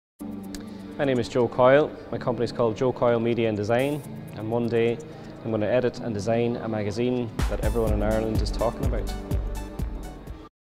Joe Coyle Media Design , Creative Entrepreneur Donegal, Ireland
Joe Coyle, Media Design, Co.Donegal exhibits his work at An Exhibition of Donegal's Creative Entrepreneurs, 2016. filmed by DNK Media Productions